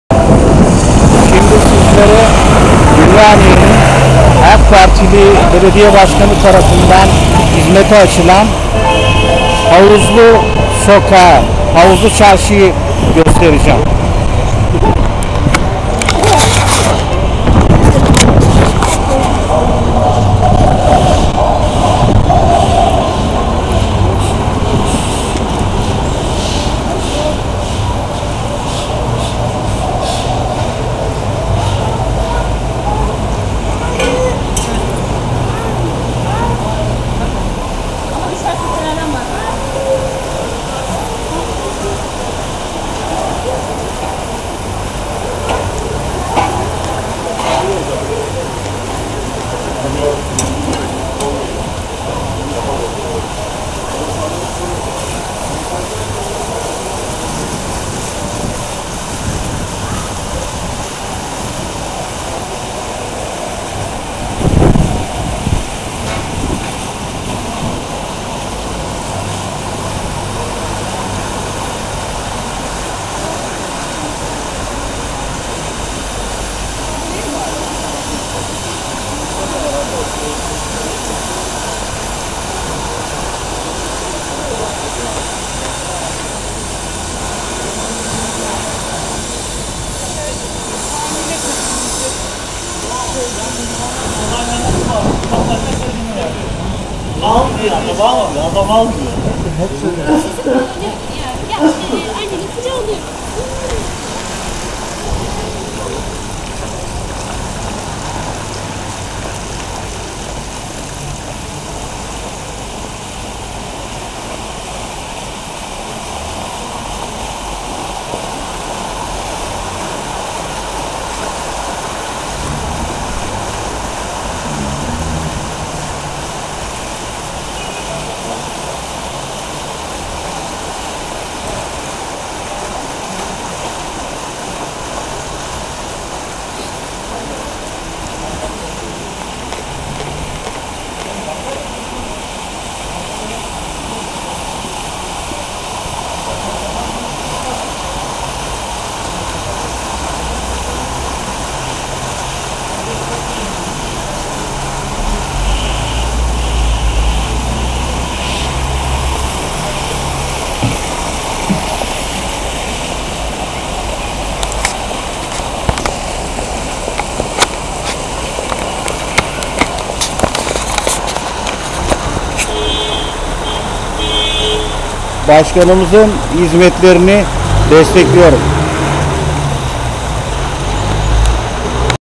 matillaatasever- ÜMRANİYE HAVUZLU ÇARŞI
Ümraniye'nin AK Partili belediye başkanı Hasan Can ilçemize havuzlu çarşıyı kazandırdı.